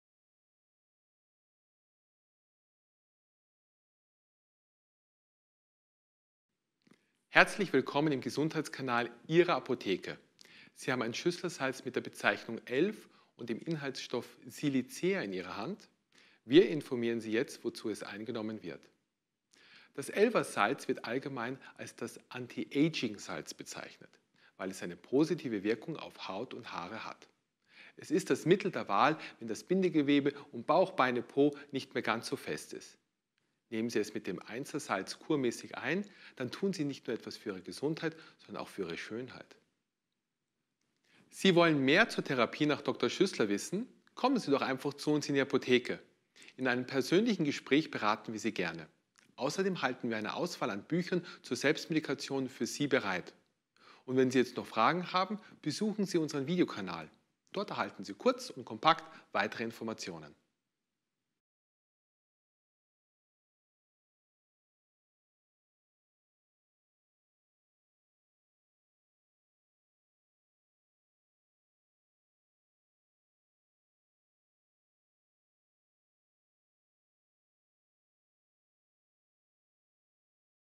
Beratungsclip Schüssler Salz Nr.11 Silicea D12
Hier berät Sie der Apotheker Thomas von Künsberg Sarre zum Anwendungsgebiet der Schüssler Salze Nr. 11